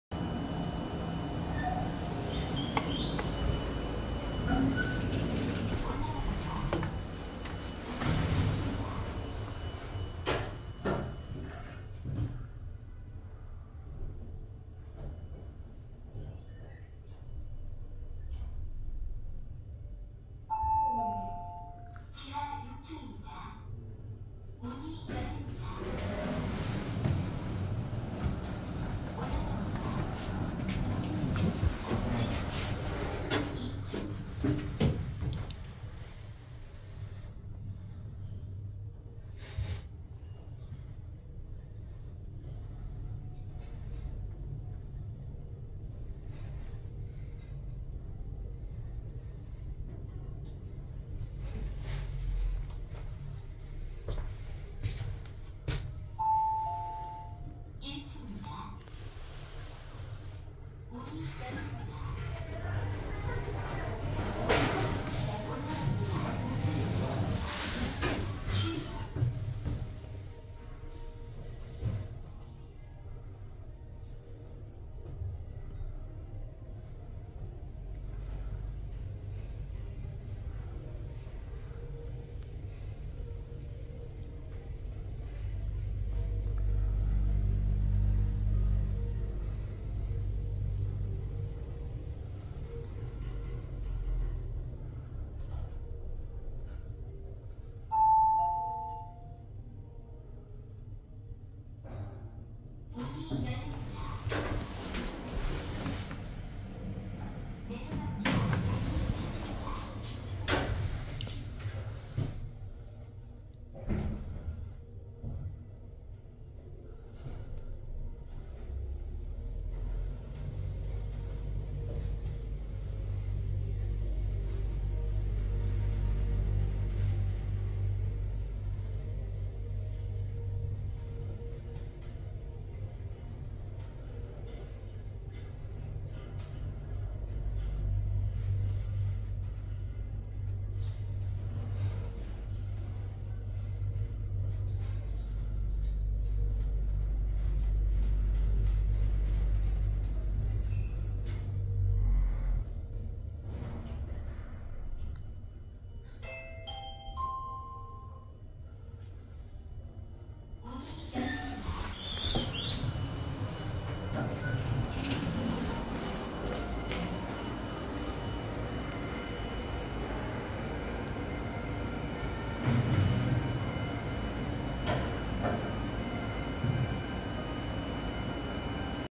경기도 성남시 분당구 미금일로154번길 20 (구미동) 2001아울렛(분당점) 나머지구간 3 구.동양엘리베이터 리모델링후 탑사기
원활한 탑사진행을 위해 B7층에 시작했으며, 오르막 도중 B6층에 승객탑승하여 1층까지 같이간점 양해바랍니다.

* 이 엘리베이터가 1차 리모델링전 1997년식 동양엘리베이터 CV60모델이었고, 2차 리모델링전 2009년식 삼일엘텍으로 추정되며, 여기는 직원들이 탈 수 있거나 오전 10시 30분 개점하자마자 바로 혼잡해질 수 있으니 탑사시 주의하십시오.